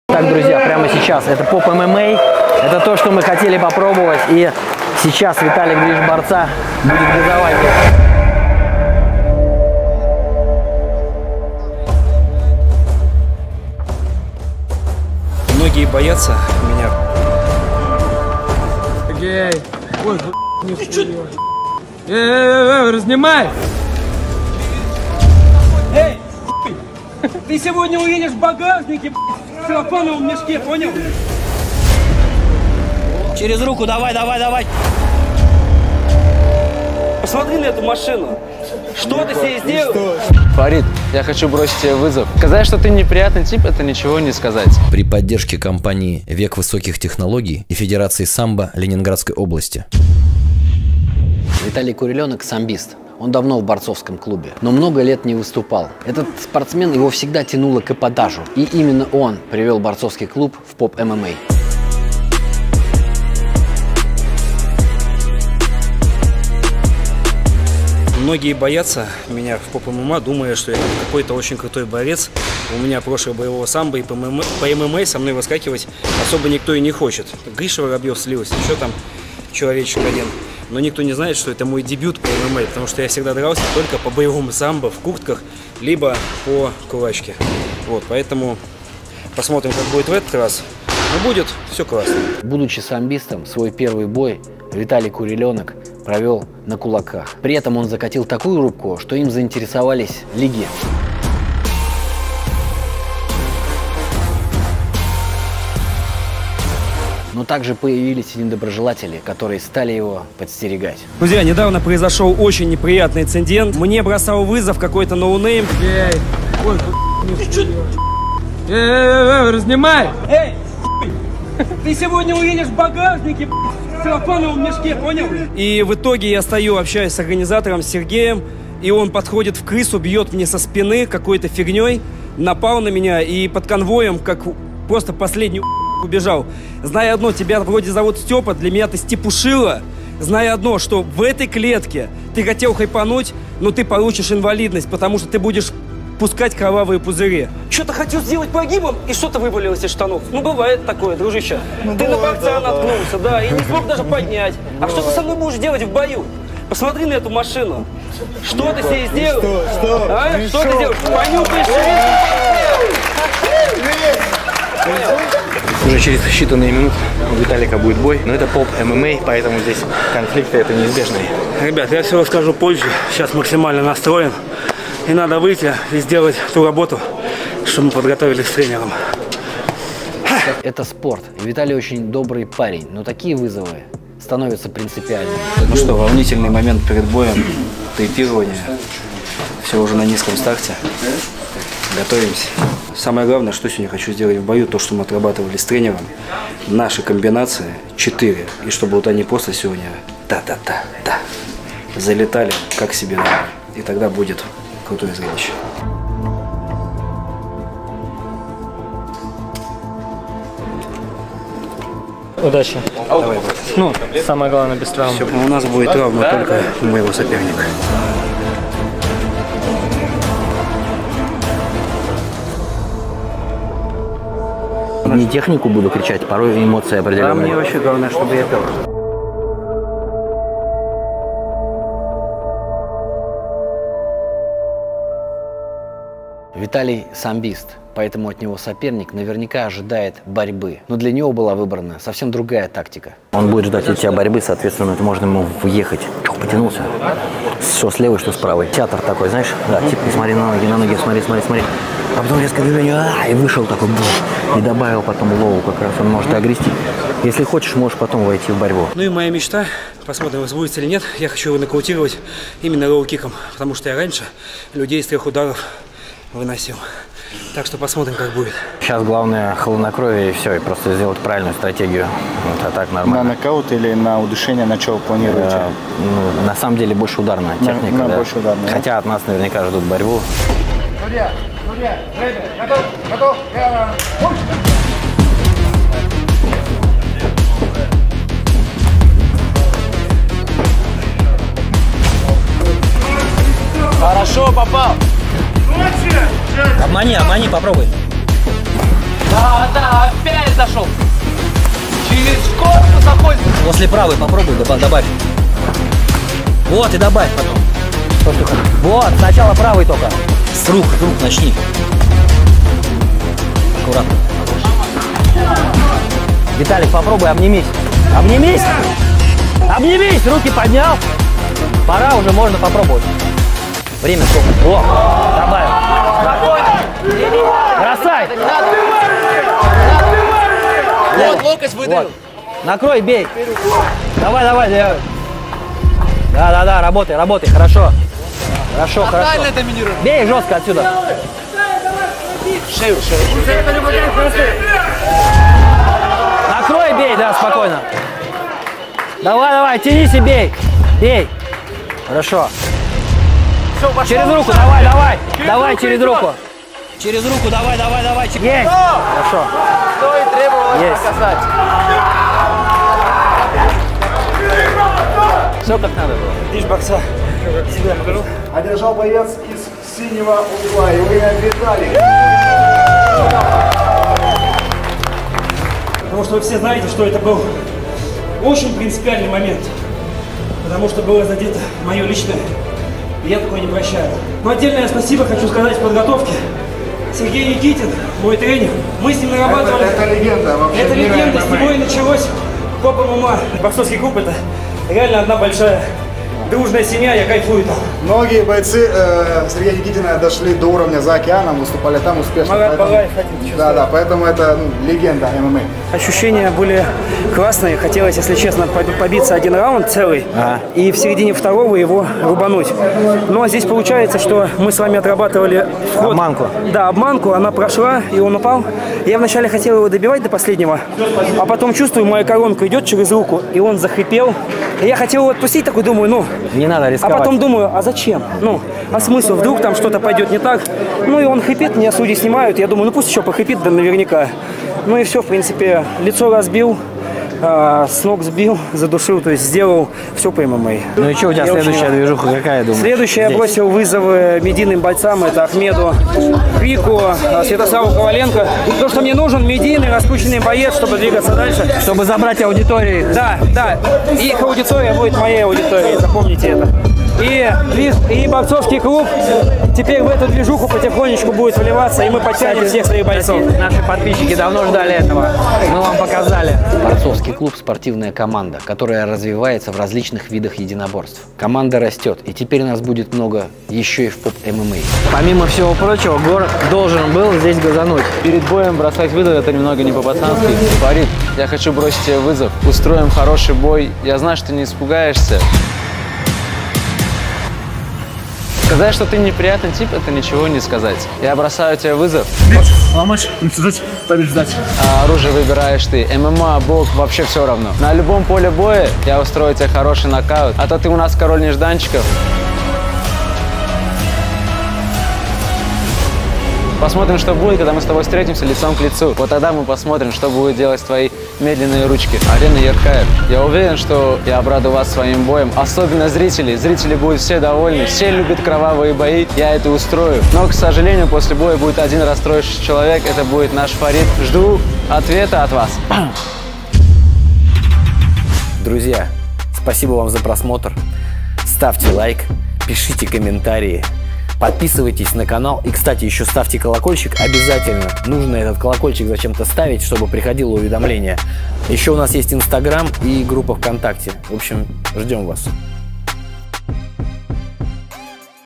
БОКСЁР НАРВАЛСЯ на САМБИСТА! Вызов ФАРИДА ЯДУЛЛАЕВА! Закулисье ARENA ERKAEV! (БК серия 27)
БОКСЁР НАПАЛ со спины На спортсмена Борцовского Клуба Виталика Движ Борца. Для Виталика это стала принципиальным противостоянием. Он жаждал оказаться поскорее с противником в клетке! Смотрите что из этого получилось!
А так же Борцовкий Клуб бросил вызов самому скандальному бойцу Фариду Ядуллаеву!
И конечно же закулисье турнира ARENA ERKAEV.

При поддержке Федерации Самбо Лен. Обл.